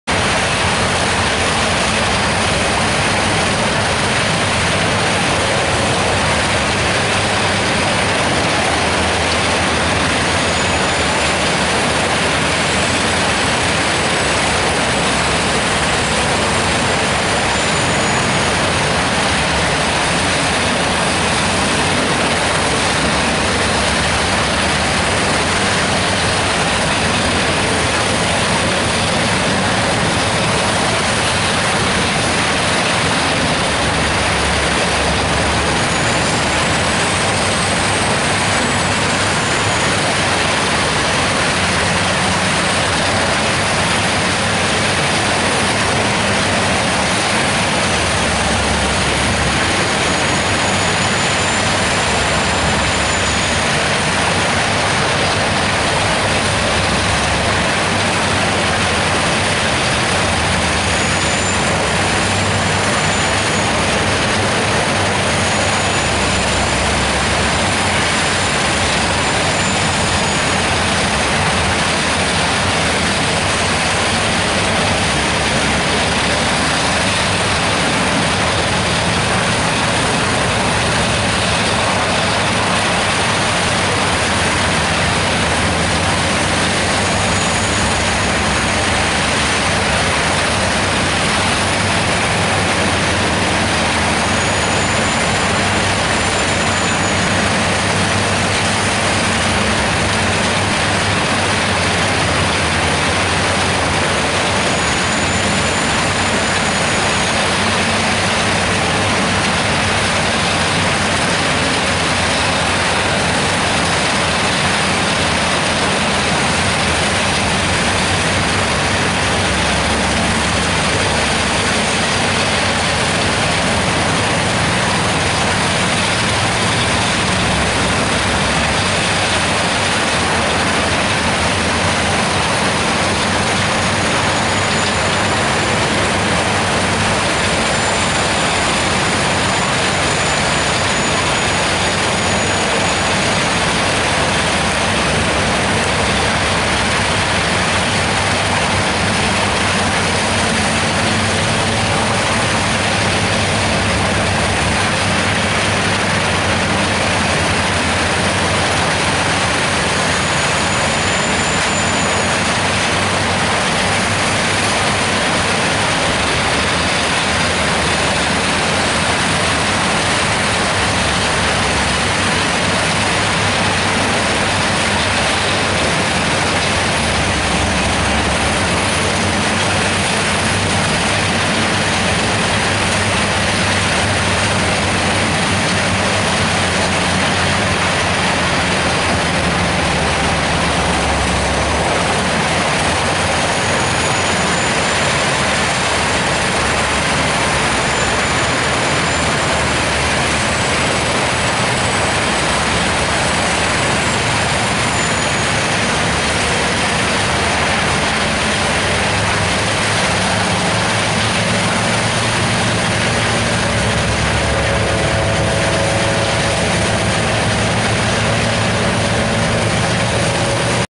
double fun
Two wood chipper in action. It might have been the chipping contest but actually it was a perform testing with round wood. All the data was gathered wirelessly and simultaniously straight from CAN-bus logger.